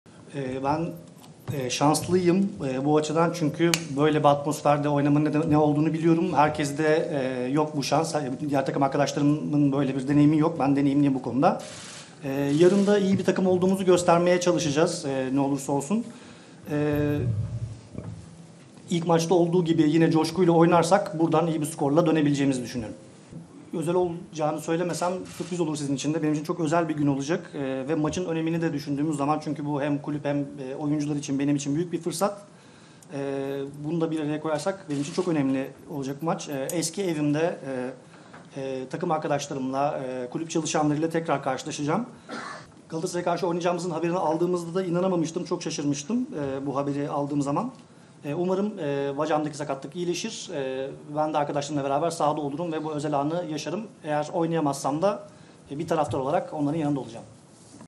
Martin Linnes'ten Galatasaraylıları duygulandıran sözler! "Benim için çok özel bir gün olacak"
Martin Linnes, Galatasaray ile eşleştiklerinde şaşırdığını dile getirerek, "Benim için çok özel bir gün olacak. Hem kulüp hem takım arkadaşlarım hem de benim için büyük bir fırsat. Çok önemli bir maç olacak. Eski evimde, takım arkadaşlarımla, kulüp çalışanlarıyla tekrar karşılaşacağım. Galatasaray'a karşı oynayacağımızın haberini aldığımda inanamamıştım, çok şaşırmıştım. Umarım bacağımdaki sakatlık iyileşir. Ben de yarın sahada olup bu özel anı yaşarım. Oynayamazsam da bir taraftar olarak onların yanında olacağım." değerlendirmesinde bulundu.